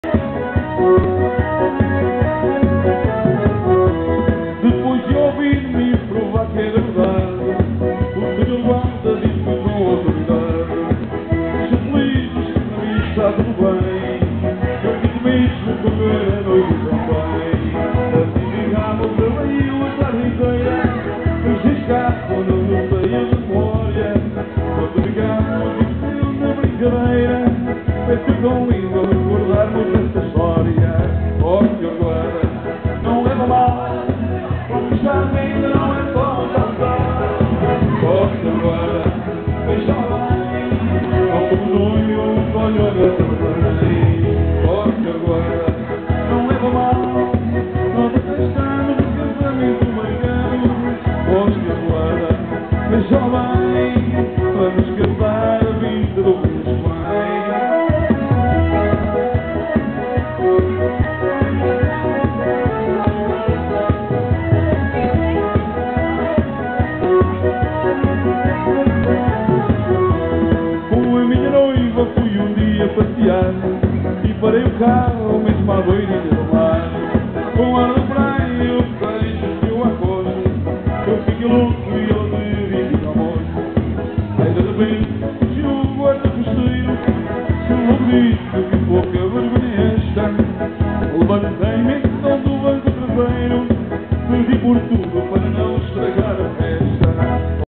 video3.dia.das.mulheres.8.03.2010.MOV
Vídeo3 - Espectáculo Realizado no Dão Catering - Treixedo Dia 8 de Março de 2010 -  Dia das Mulheres. Foi mais um Espectáculo.